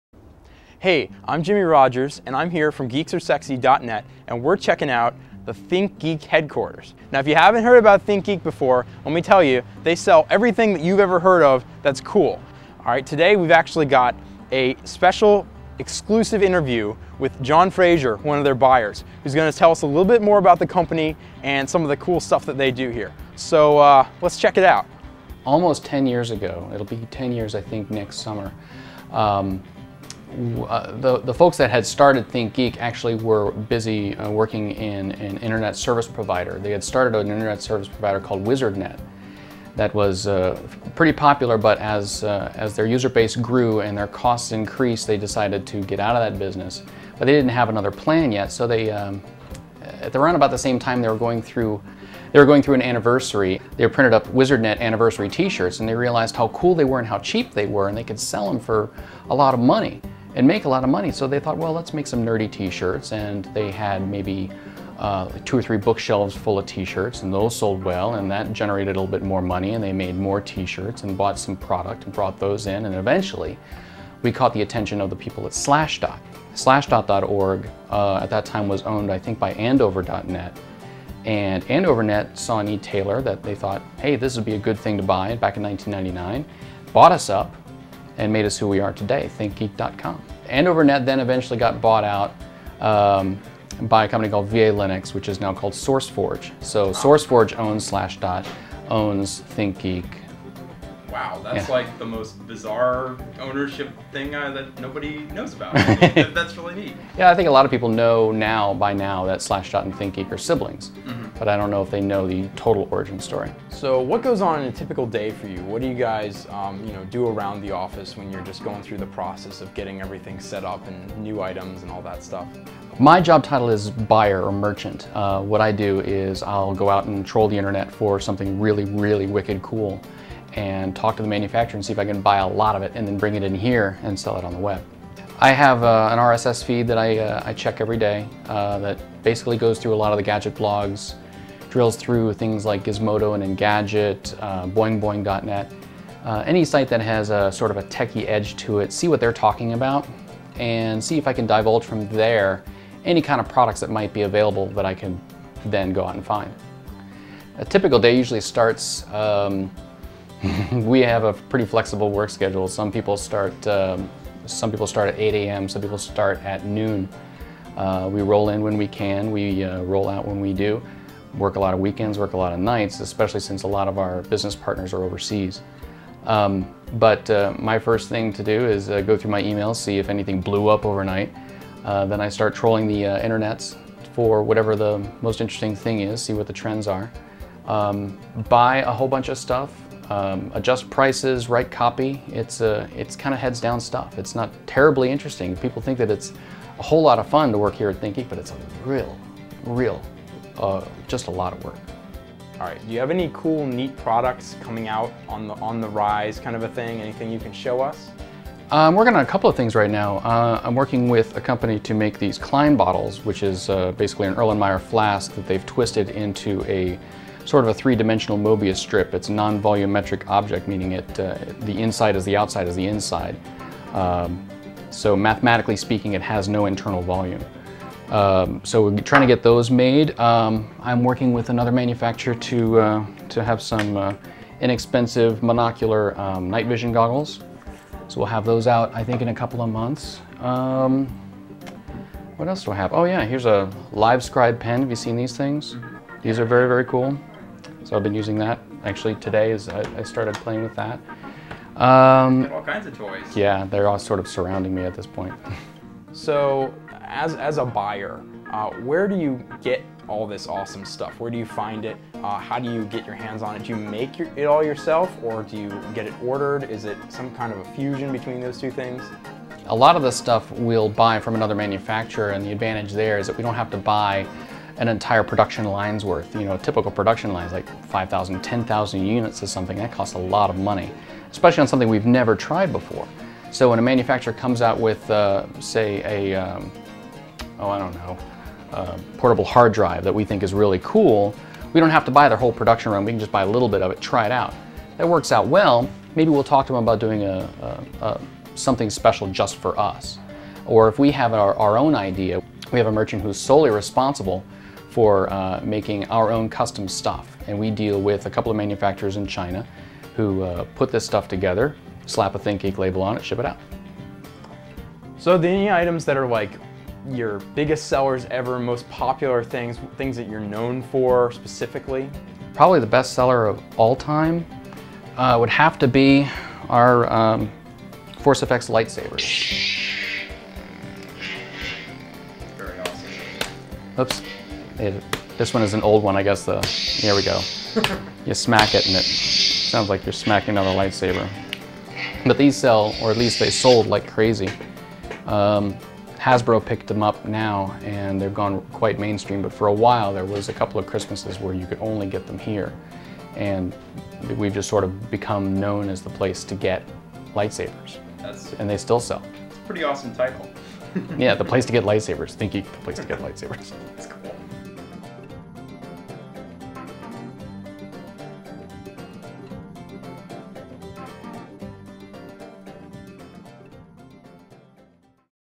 Inside ThinkGeek - The Interview V.2
Here is an exclusive look inside ThinkGeek's headquarters. Jimmy Rogers, our field correspondent at Geeks are Sexy , grabbed an interview with John Frazier, one of ThinkGeek's buyers who finds all their neat stuff.

More information about the interview can be found as well as on Jimmy's Blog